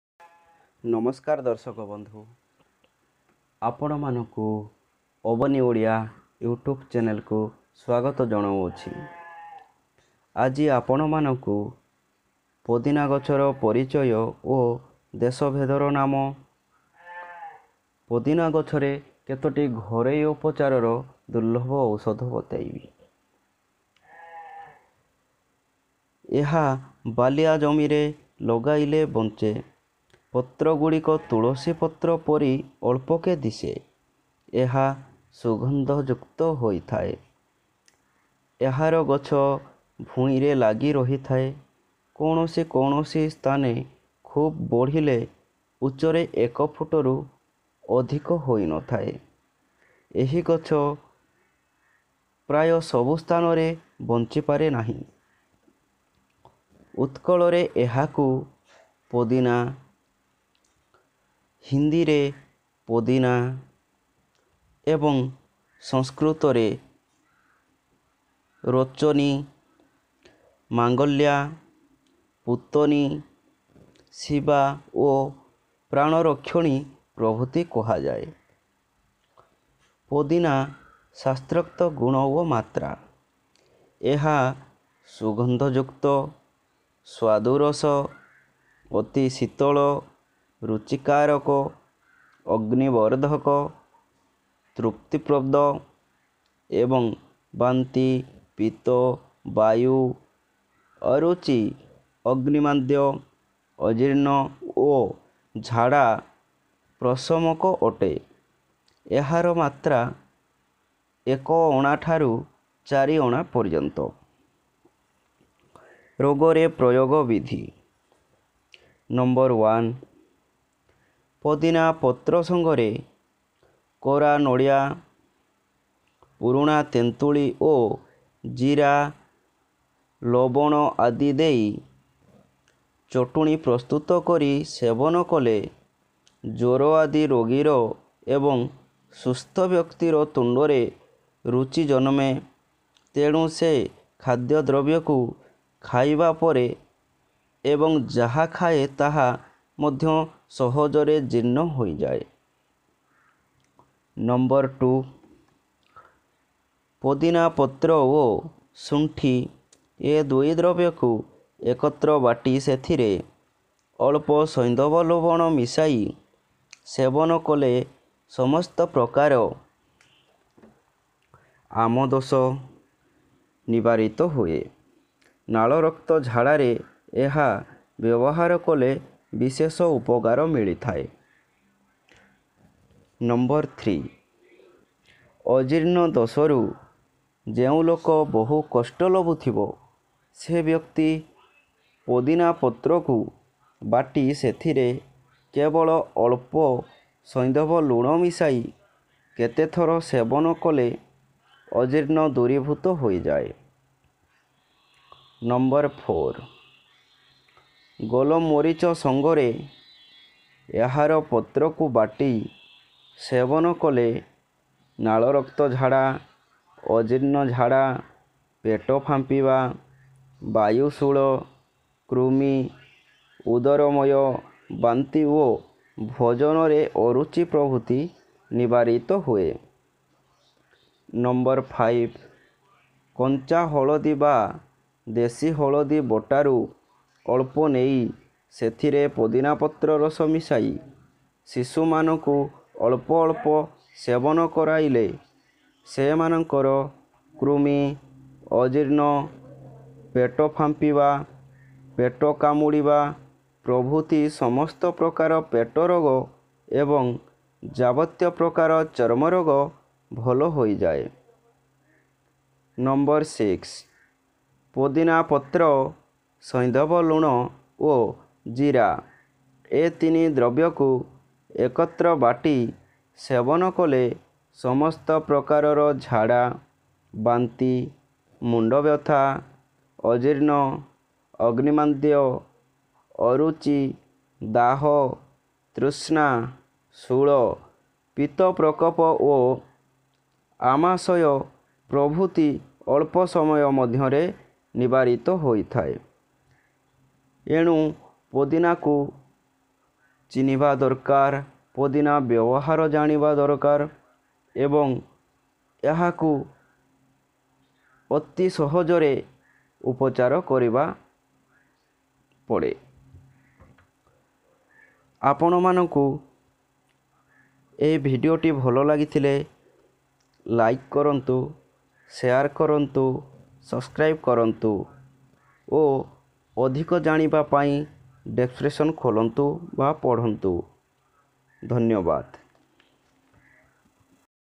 Abani odia ପୋଦିନା ଗଛର ଉପକାରିତା ଓ କେତଟୀ ଦେଶି ଉପଚାର
ପୋଦିନା ଗଛର ଔଷଧ ବିଷୟରେ
1  ପୋଦିନା ପତ୍ର ସଙ୍ଗରେ କୋରା ନଡ଼ିଆ, ପୁରୁଣା ତେନ୍ତୁଳି ଓ ଜିରା ଲୁଣ ଆଦି ଦେଇ ଚଟୁଣୀ ପ୍ରସ୍ତୁତ କରି ଖାଇଲେ ଜ୍ୱର ଆଦି ରୋଗିର ଏବଂ ସୁସ୍ଥ ବ୍ୟକ୍ତି ର ପାଟି ରୂଚି କର ହୋଇଥାଏ ସେ ଖାଦ୍ୟ ଖାଇପାରେ ଏବଂ ଖାଦ୍ୟ ଭଲଭାବରେ ହଜମ ହୋଇଥାଏ
2  ପୋଦିନା ପତ୍ର ଓ ଶୁଣ୍ଠି ଏଦୁଇ ଦ୍ରବ୍ୟ କୁ ଏକତ୍ର ବାଟି ସେଥିରେ ସୈନ୍ଧବ ଲବଣ ମିଶାଇ ସେବନ କଲେ ସମସ୍ତ ପ୍ରକାର ଆମଦୋଷ ନିବାରିତ ହୁଏ ନାଳ ରକ୍ତ ଝାଡ଼ା ରେ ବ୍ୟବହାର କଲେ ବିଶେଷ ଉପକାର ମିଳେ
3  ଅଜିର୍ଣ୍ଣ କାରଣରୁ ଯେଉଁ ଲୋକ କଷ୍ଟ ଲଭୁଥିଲେ ସେ ବ୍ୟକ୍ତି ପୋଦିନା ପତ୍ର କୁ ବାଟି ସେଥିରେ ଅଳ୍ପ ସୈନ୍ଧବ ଲବଣ ମିଶାଇ କେତେଥର ସେବନ କଲେ ଅଜୀର୍ଣ ଦୂରିଭୁତ ହୋଇଥାଏ
4   ଗୋଲମରିଚ ସଙ୍ଗରେ ଏହାର ପତ୍ର କୁ ବାଟି ସେବନ କଲେ ପେଟ ଫାମ୍ପିବା ବାୟୁ ଶୂଳ ବାନ୍ତି ରୋଗ ନିବାରିତ ହୁଏ
5   କଞ୍ଚା ହଳଦୀ ବା ଦେଶୀ ହଳଦୀ ବଟା ରୁ ଅଳ୍ପ ନେଇ ସେଥିରେ ପୋଦିନା ପତ୍ର ରସ ମିଶାଇ ଶିଶୁ ମାନଙ୍କୁ ଅଳ୍ପ ଅଳ୍ପ ସେବନ କରାଇଲେ ସେମାନଙ୍କର କୃମି ପେଟକାଟିବା ଯାବତୀୟ ପ୍ରକାର ଚର୍ମରୋଗ ଭଲହୁଏ
6  ପୋଦିନା ପତ୍ର ଓ ସୈନ୍ଧବ ଲବଣ ଜିରା ଏତିନି ଦ୍ରବ୍ୟ କୁ ଏକତ୍ର ବାଟି ସେବନ କଲେ ସମସ୍ତ ପ୍ରକାର ଝାଡ଼ା ବାନ୍ତି ମୁଣ୍ଡବ୍ୟଥା ଅଜୀର୍ଣ ଶୂଳ ପିତ୍ତ ପ୍ରକୋପ ଓ ଅମାଶୟ ପ୍ରଭୁତି ଅଳ୍ପ ସମୟ ଭିତରେ ଭଲହୁଏ